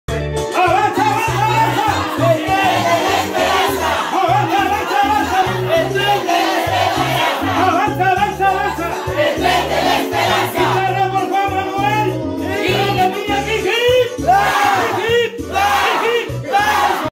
Avanza País.
Seguimos recorriendo nuestra querida provincia de San Martín. Reiniciamos nuestra campaña 2022, San Antonio de Cumbaza junto a nuestro candidato distrital Roque Piña Amasifuen.